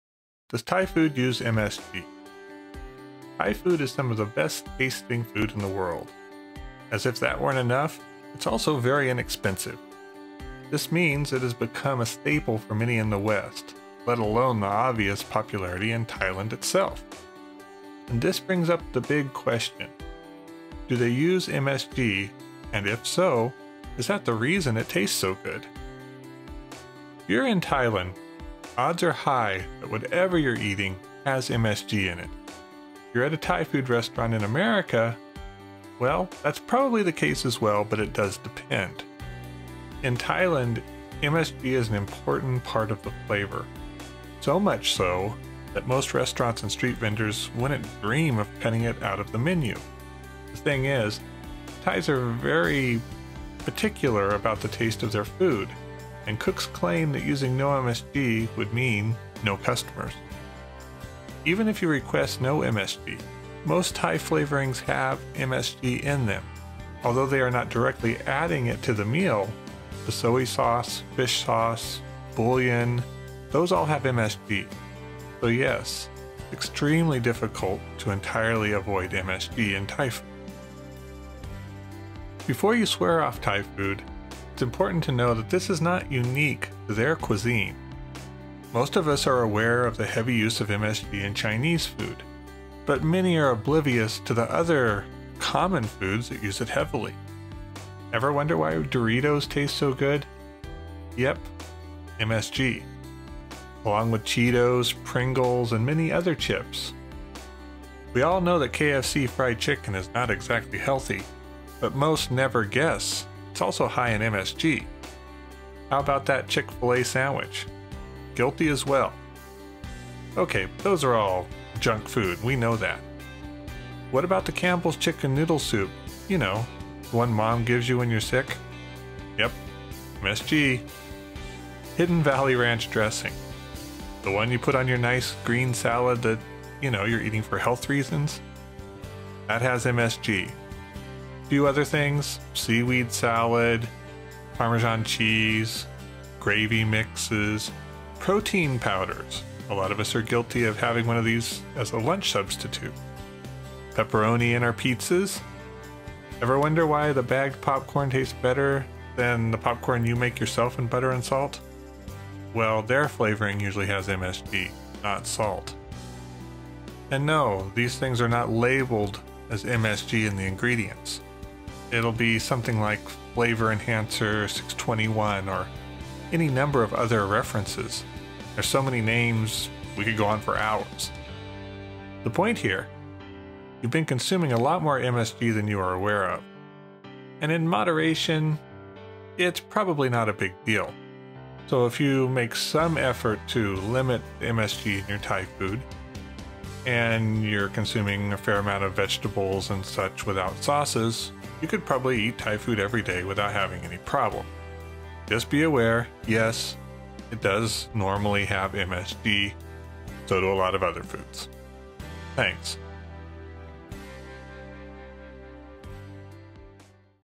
does Thai food use MSG in it? thai cooking monosodium glutamate in thailand
Does Thai food use MSG? The short answer is YES. Thai food has MSG in it. At least that is true most of the time. Watch the video to learn more. Monosodium glutamate is a flavor enhancer. Is Thai Food Healthy? It depends on the dish, but in general, it is significantly more healthy than Chinese food.  Bangkok thailand is filled with wonderful street food. Just be aware that you will be eating MSG.